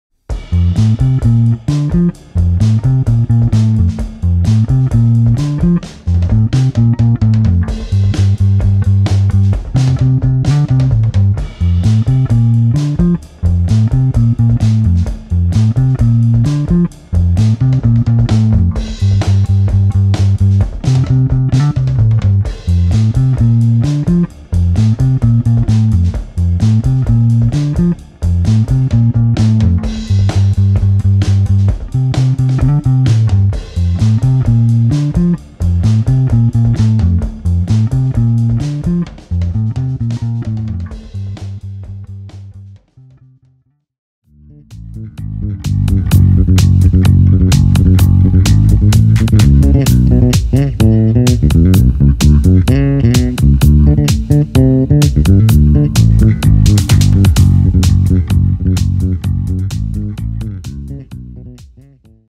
R&B Bass Riff of the day in 30sec D#268
Giorno 268, Oggi vi suono un riff R&B enjoy! :)

Il basso utilizzato è un Fender Precision del 1977 info e foto